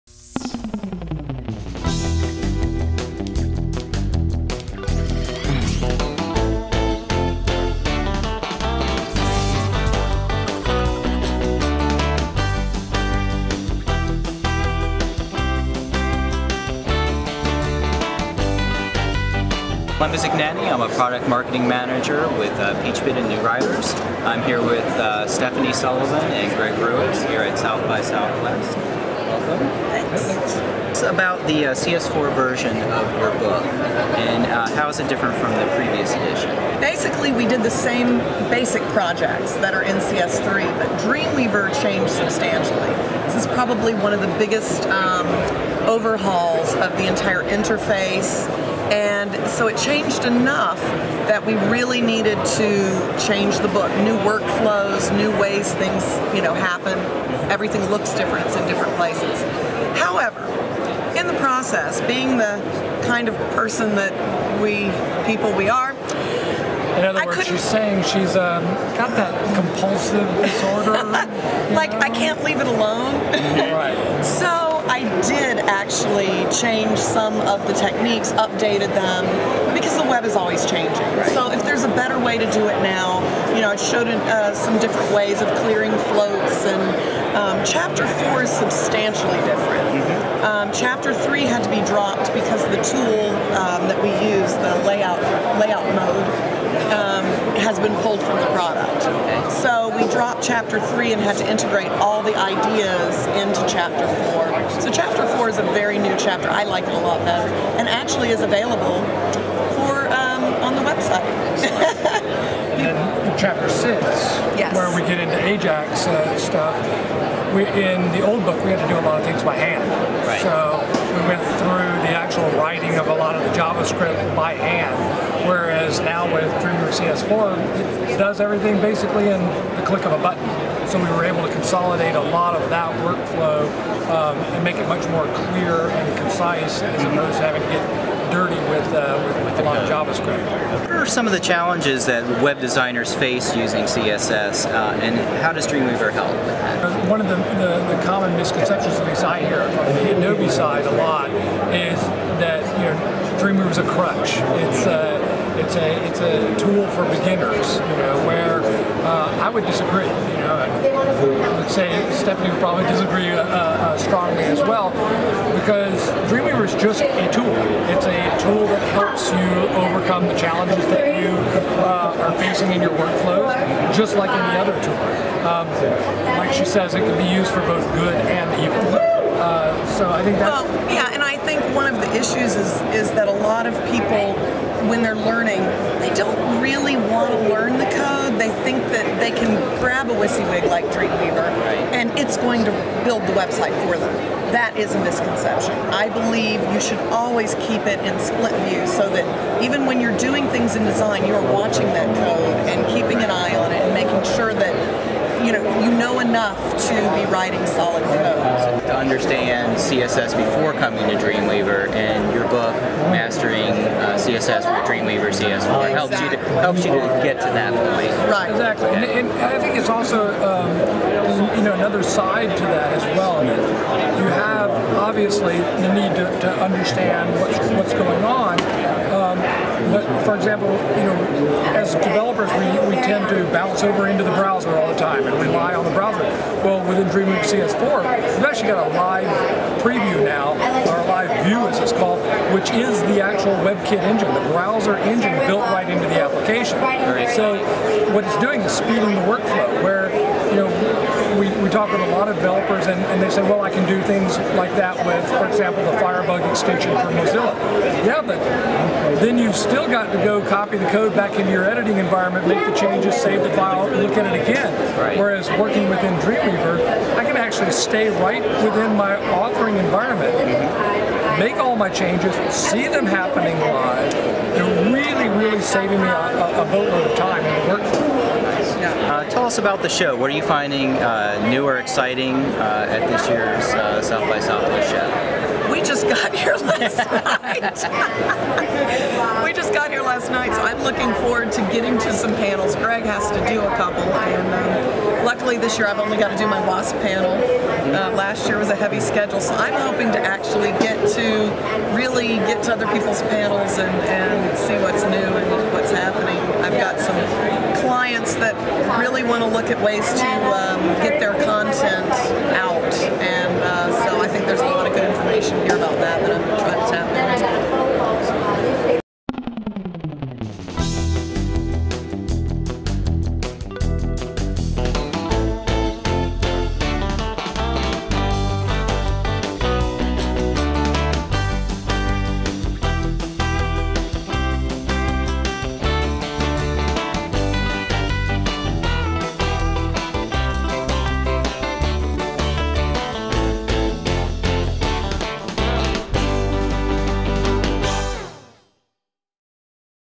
Peachpit TV: SXSW 2009 Stephanie Sullivan and Greg Rewis
SXSW 2009 interview with Stephanie Sullivan and Greg Rewis in the New Riders booth. Stephanie and Greg talk about their new book: Mastering CSS with Dreamweaver CS4.